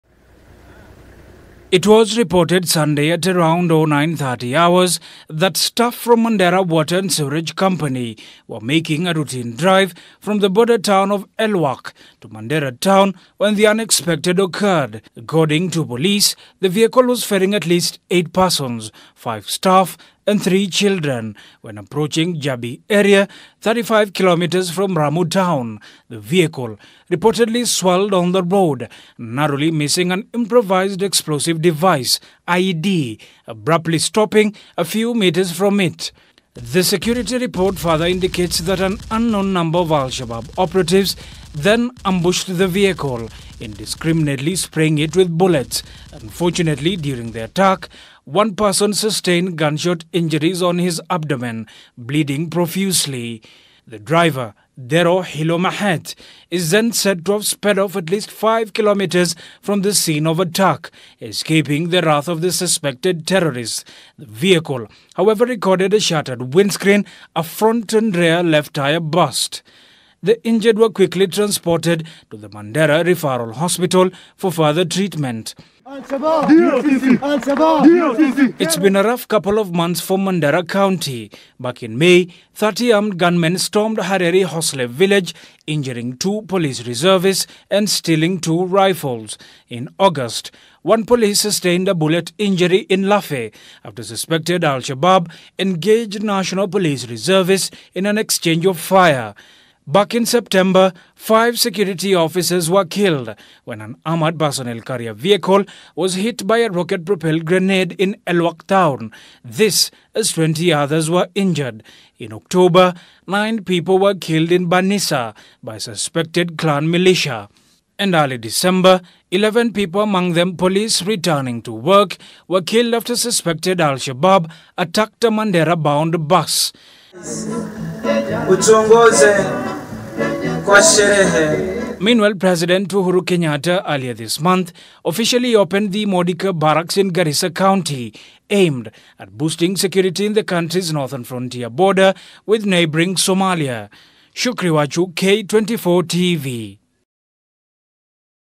Mandera foiled attack
A manhunt has been launched for suspected Al-shabab operatives following an attempted IED attack on a land cruiser ferrying five Mandera water and sewerage company staff, as well as three children traveling from Elwak to Mandera town.
 The incident which occurred at around 9:30 am Sunday, saw the driver narrowly escape the IED before the unknown number of Al-shabab operatives sprayed the vehicle with bullets, injuring one of the staff before the driver maneuvered his way, driving five kilometers from the scene of attack.